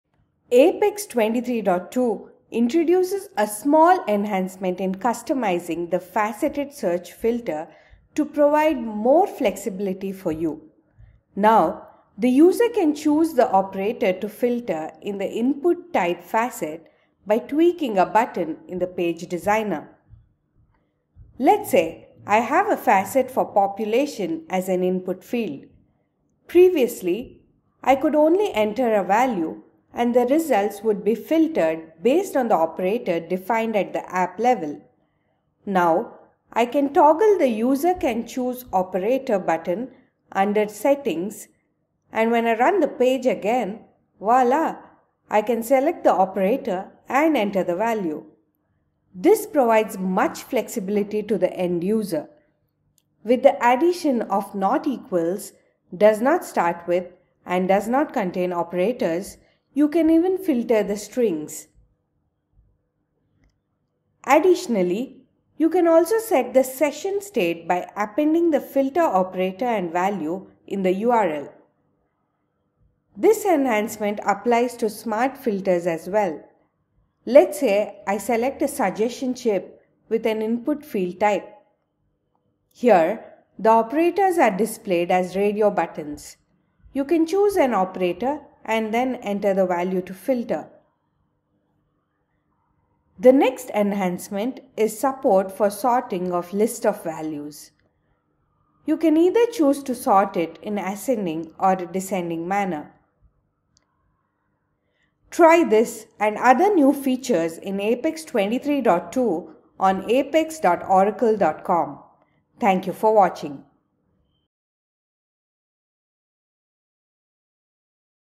Faceted Search and Smart Filters Enhancements in Oracle APEX 23.2
APEX 23.2 introduces enhancements in customizing the faceted search and Smart Filters filter to provide more flexibility for you. The end user can now select the type of operator and value to filter in the input type facet. You can also choose the sorting order for distinct type of List of Values.